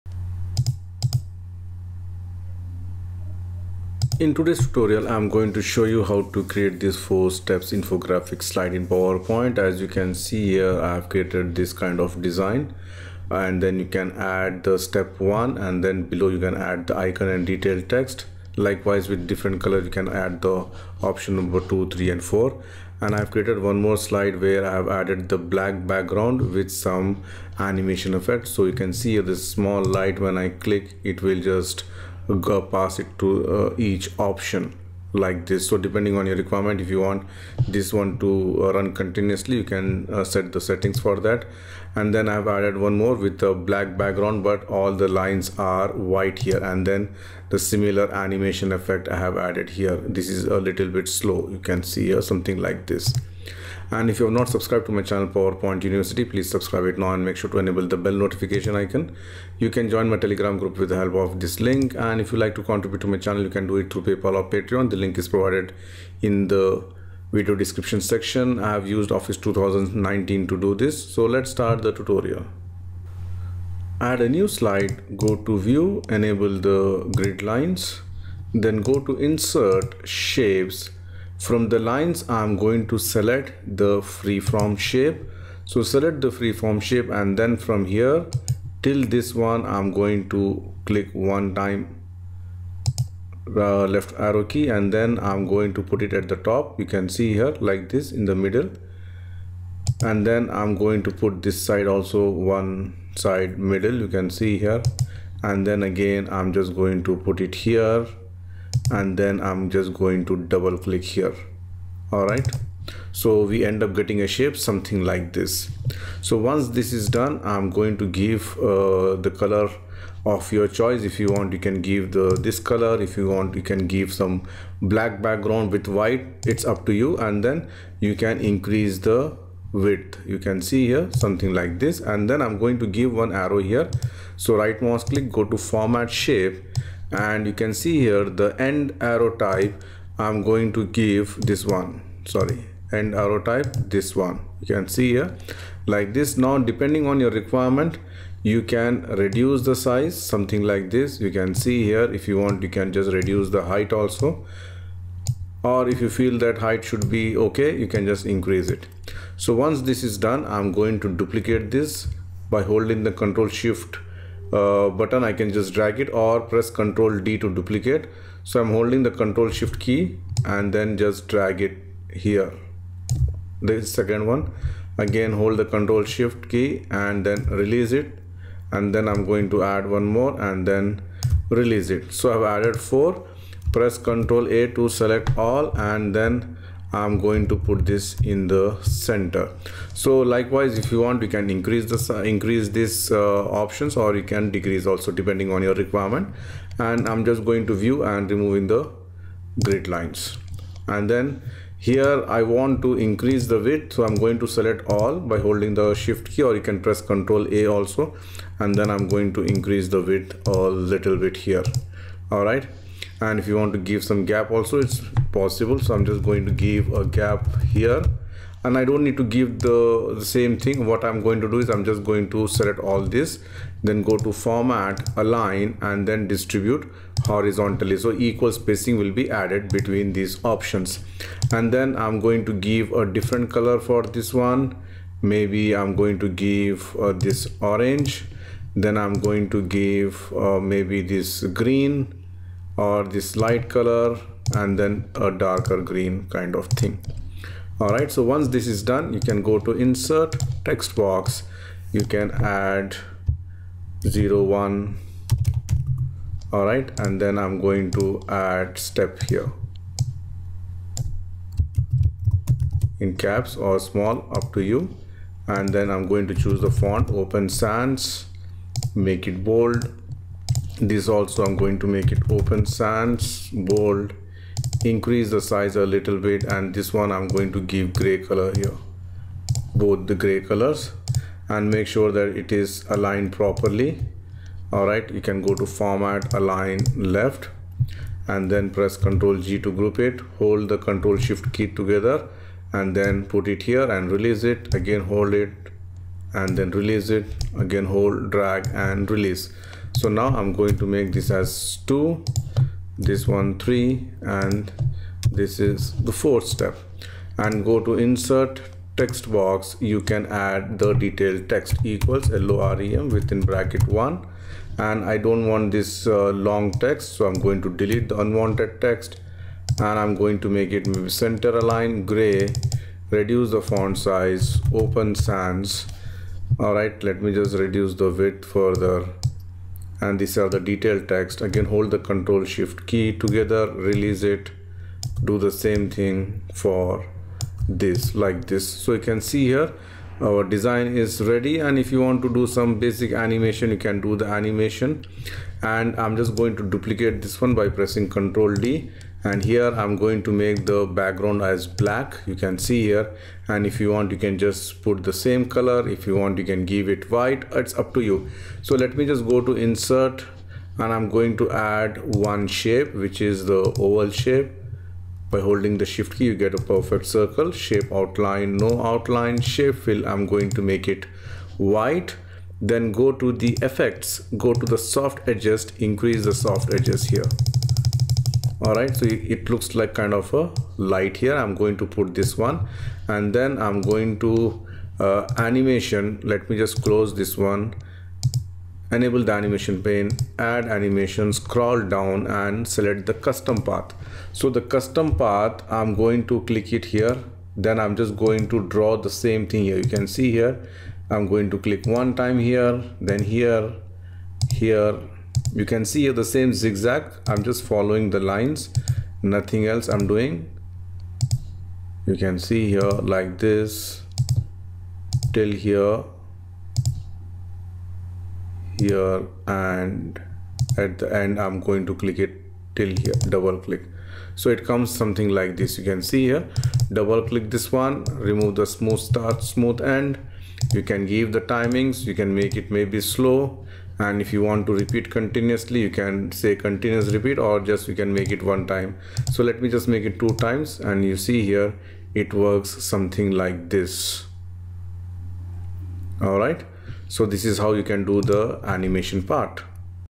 Create 4 Step Lifeline Shape Infographic Slide in PowerPoint - Tutorial No. 836
(🚫This is copyright content and cannot be uploaded or reproduced on YouTube without the consent from the content owner, You are free to use for personal purposes)